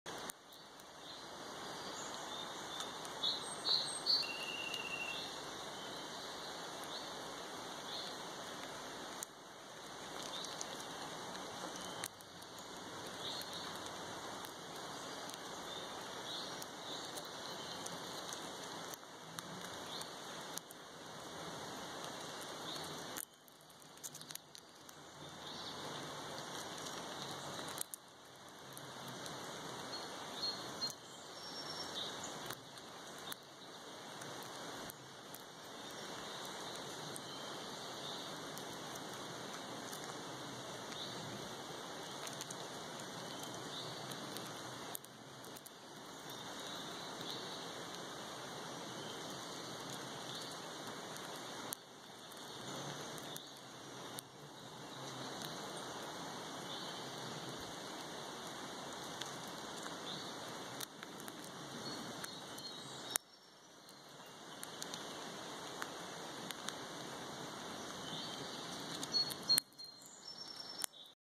Red Squirrel in 4K Ultra HD
Extreme 4K UHD close-up of a friendly Red Squirrel eating some sunflower seeds. He doesn't mind if you watch him eat just  don't expect to have any seeds!!